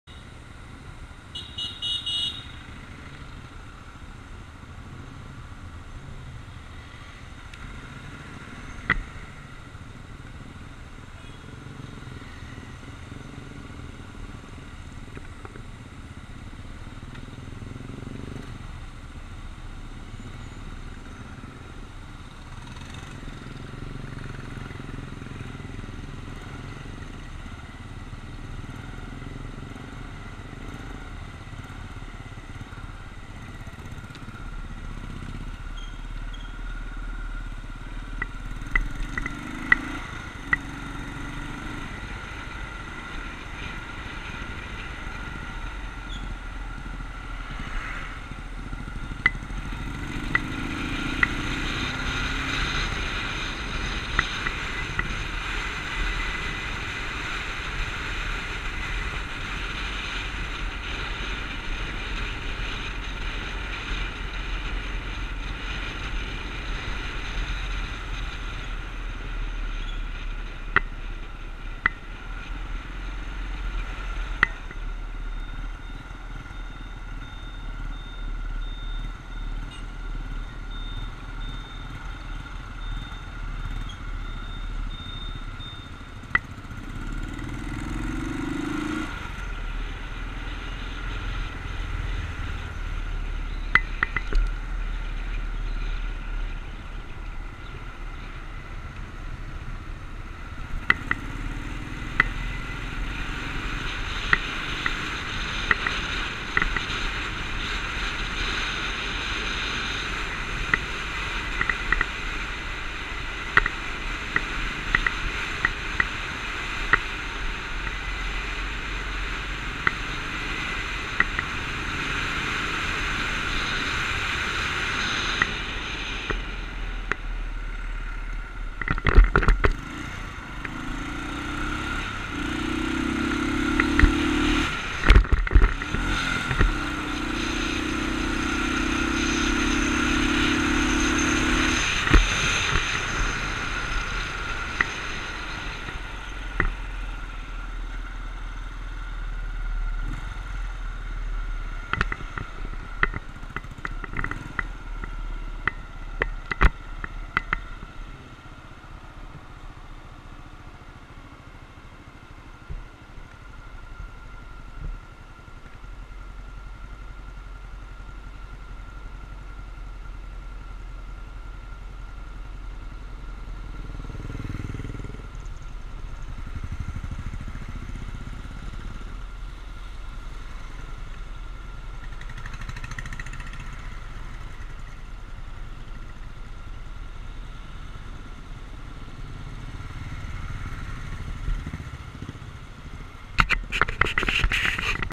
Sikkim Riders Club 70th Independence Day Ride Glimpses- 4
Sikkim Riders Club is a non profit non governmental motorcycling club based in Gangtok Sikkim and catering to diverse riders from different aspects. They ride for a cause and ride to inspire. This is a glimpse of the ride undertaken from Gangtok to Namchi Blind School on the occassion of the 70th Indian Independence Day.